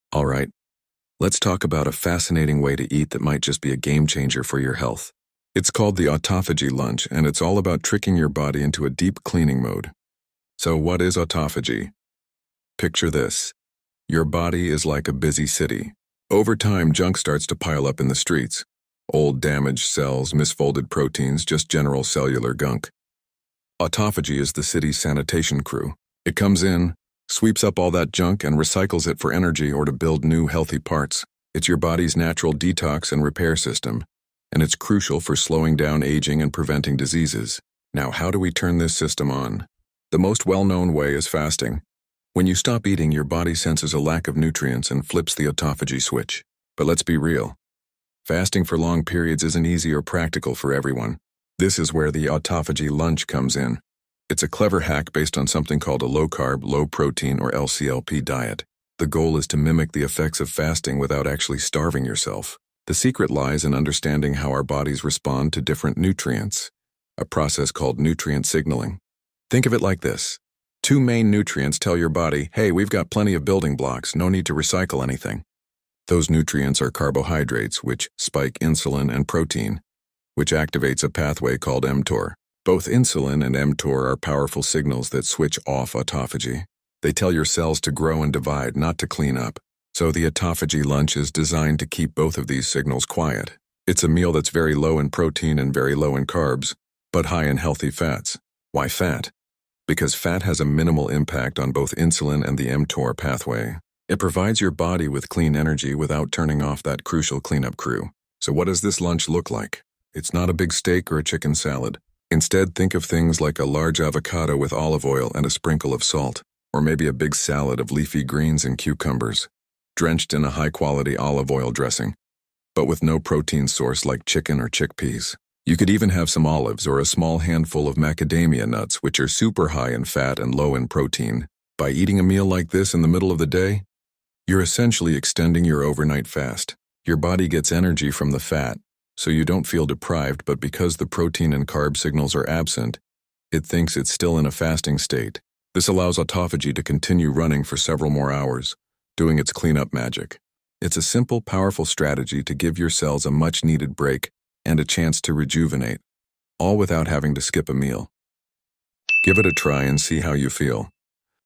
The "Autophagy Lunch" for Cellular Renewal & Anti-Aging (The Ultimate Cellular Detox)
🌟 The Autophagy Lunch: The 4-Hour FASTING HACK to Clean Your Cells (Without Starving!)
Did you know you can trick your body into a deep-cleaning, cellular repair mode without skipping a meal? This video dives deep into the science behind the "Autophagy Lunch"—a simple, powerful strategy to naturally detox your body and boost longevity.
The secret? A specific Low-Carb, Low-Protein (LCLP) meal designed to keep the fasting switch ON. We'll explain exactly how this fat-focused lunch bypasses the two main signals (Insulin and mTOR) that stop your body's essential cleanup process, Autophagy.
🔑 In This Video, You Will Learn:
What is Autophagy? (The body's natural cellular recycling and anti-aging mechanism).
The Science of Nutrient Signaling: Why carbs and protein stop Autophagy (mTOR and Insulin explained).
The Autophagy Lunch Blueprint: The exact macro breakdown (Low-Carb, Low-Protein, High-Fat) that mimics a fast.
Easy Meal Ideas: Specific examples of what an Autophagy Lunch looks like (Avocado, Olive Oil, Olives, etc.).
The Benefits: How this simple hack can support weight loss, boost energy, and slow down cellular aging.
🥗 Autophagy Lunch Examples (What to Eat)
A large avocado drizzled with high-quality extra virgin olive oil and sea salt.
A large serving of leafy greens (spinach, arugula) and cucumber with an oil and vinegar dressing (no protein!).
A small handful of macadamia nuts or olives.